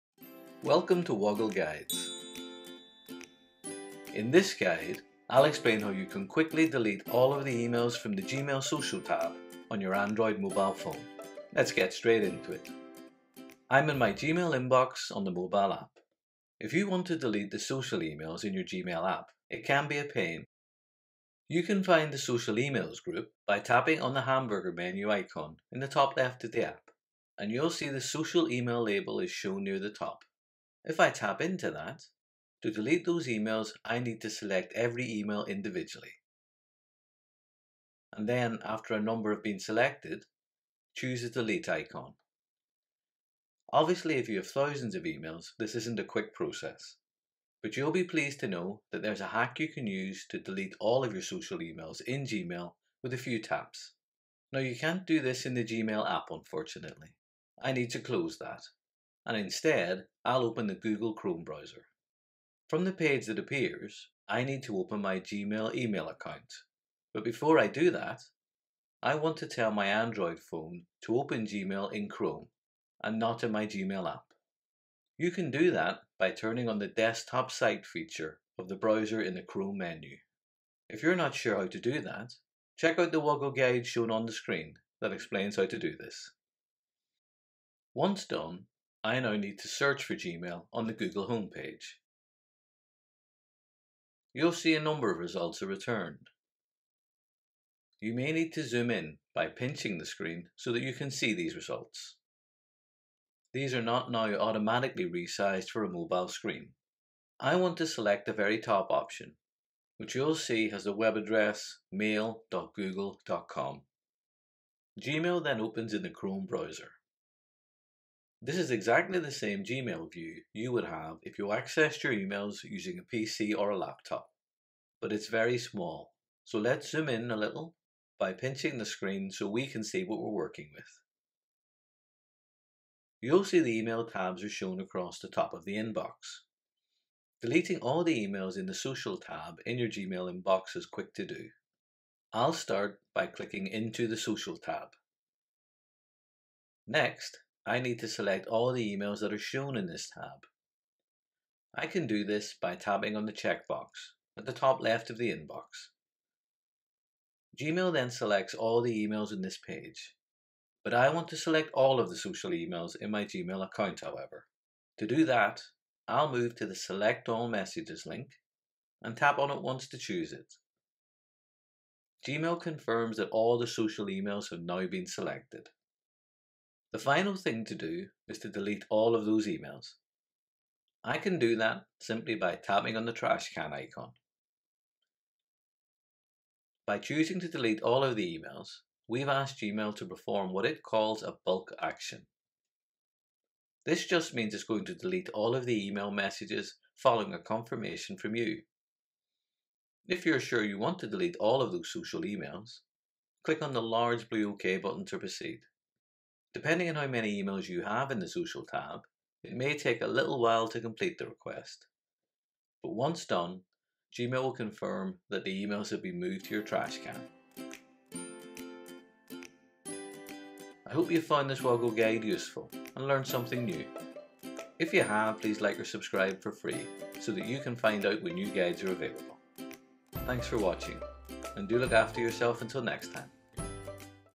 How to delete social emails in Gmail Mobile app
In this guide, I’ll explain how you can quickly delete all of the social emails in Gmail on the Gmail mobile app

🕔  Key Moments
00:00 | Introduction
00:13 | How to delete social emails in Gmail
03:40 | Wrap up